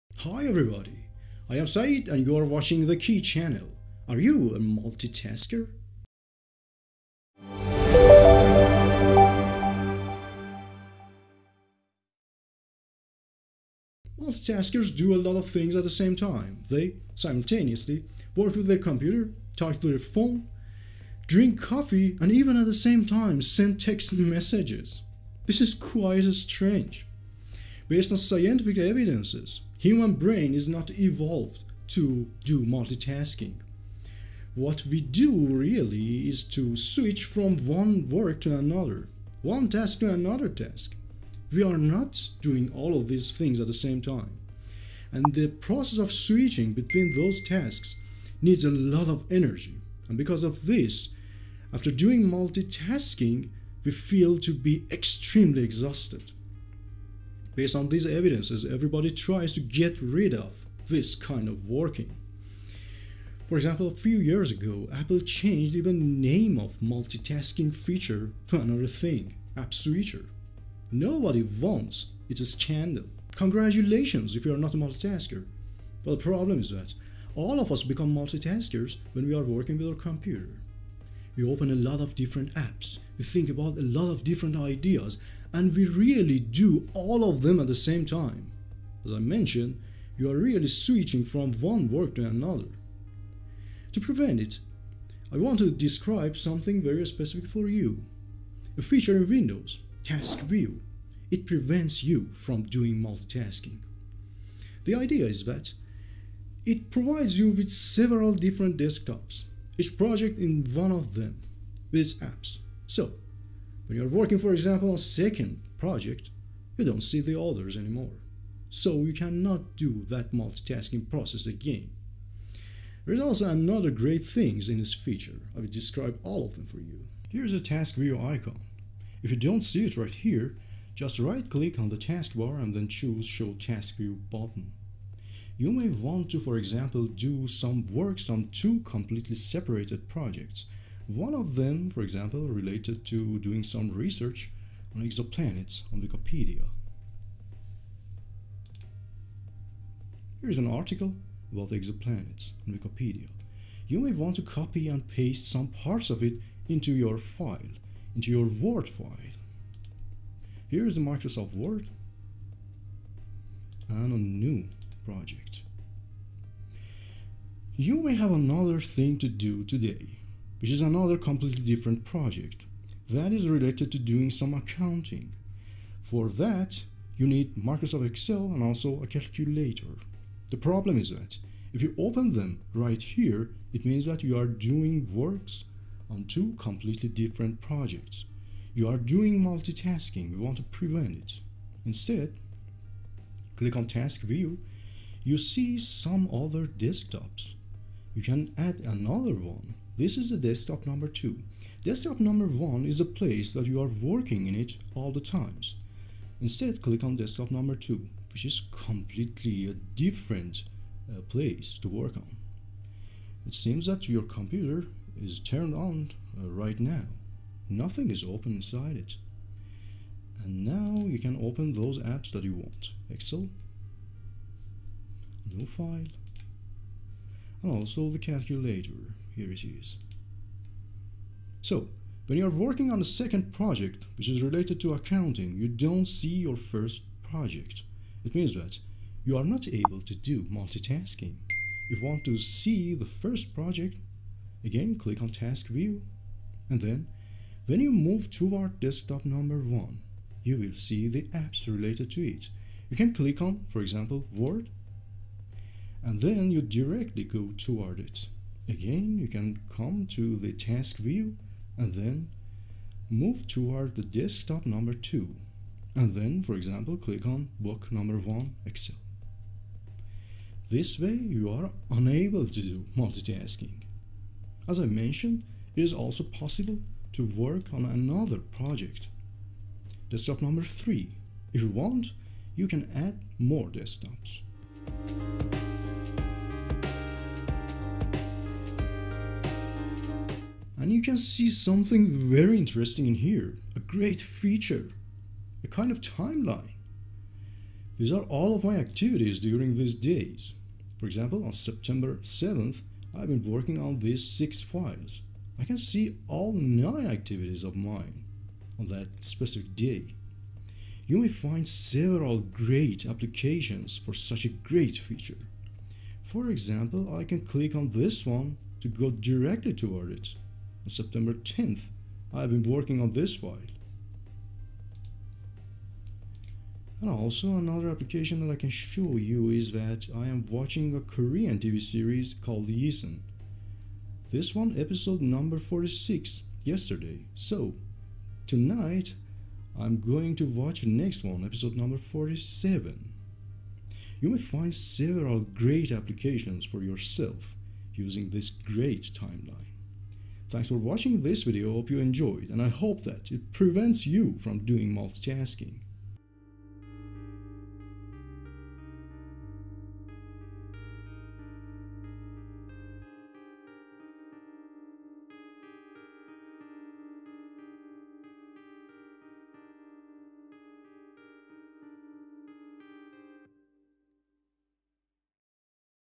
How Windows Task View Prevents You From Multitasking
Windows Task View provides you with several virtual desktops, enabling you to separate your projects when working with your computer / laptop. Separating projects will lead to more focus and above all, hold you back from Multitasking.

In this video I will tell you why multitasking is harmful in general, and how to protect yourself from it using Windows Task View when you use your computer / laptop. Then, I will describe Windows Timeline!

Saeed